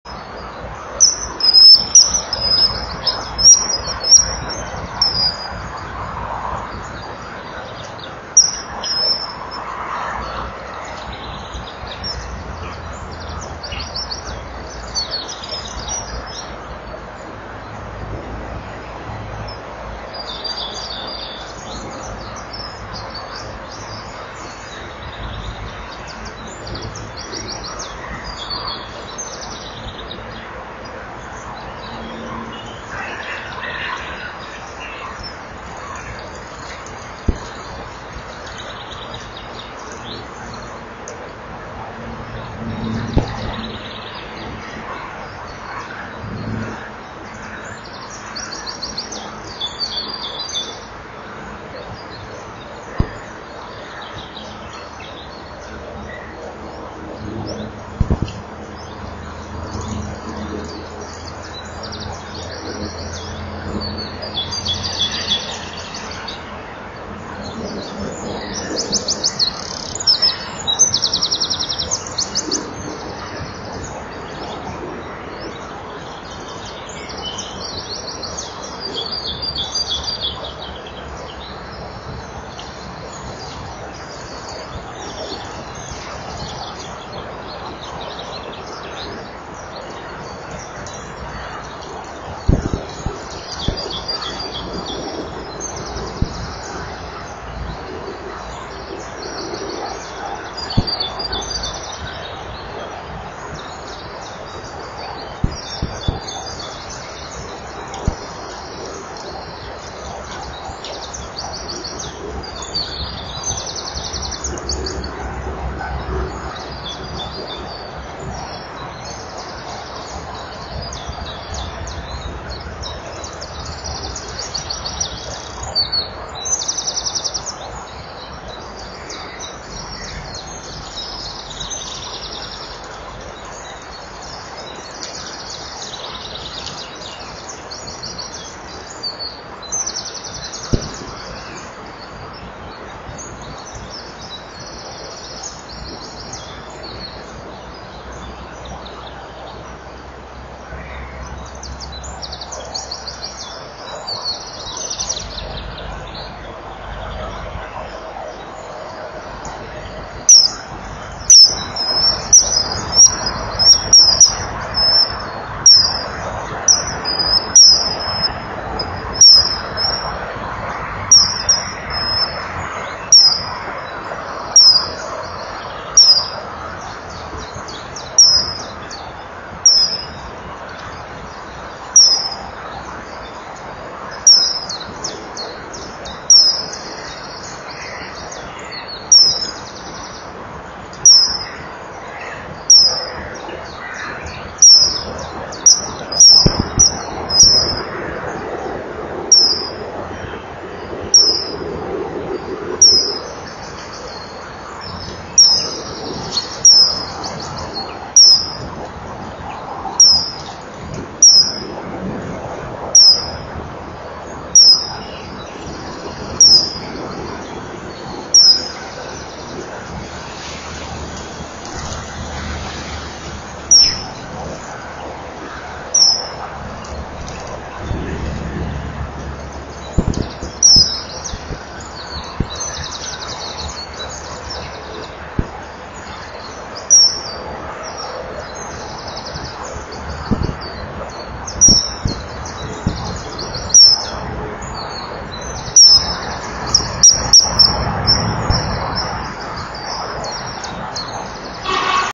broedwandcam-2012-04-18-111402_IJ_M_voor_later_V_uit_gang_erbij_M_achter_in_gang_V_weg.wmv
11.14 uur. IJsvogel M komt voor. Hij moet even geduld hebben, maar daar komt V uit de gang en ook op de stok voor. Hij maakt plaats en is even uit beeld. Kennelijk nog even een duik of een visje proberen te vangen en hij zit achter. Hij gaat met een sierlijke boog de gang in. V piept nog wat na en vertrekt om zelf op zoek te gaan naar vis.